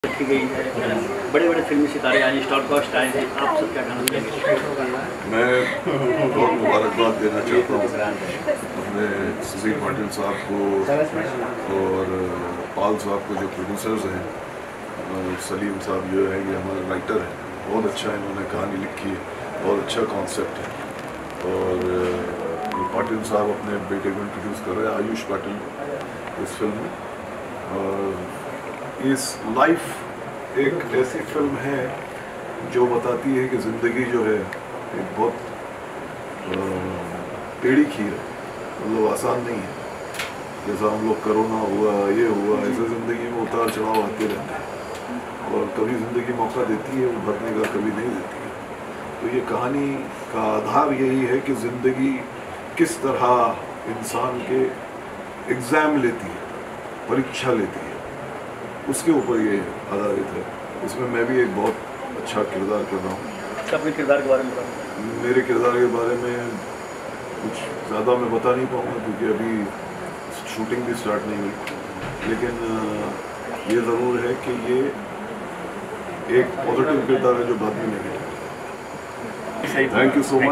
Musical Muhurat Of Hindi Movie The LIFE Singer MOH. Salamat, Producer Sudhir Patil & Gaurav pal
Musical Muhurat Of Hindi Movie "The LIFE" Singer MOH. Salamat, Producer Sudhir Patil & Gaurav pal,  Music Afroz Khan, cast- Ayush patil, Shahbaz Khan, Riya Kapoor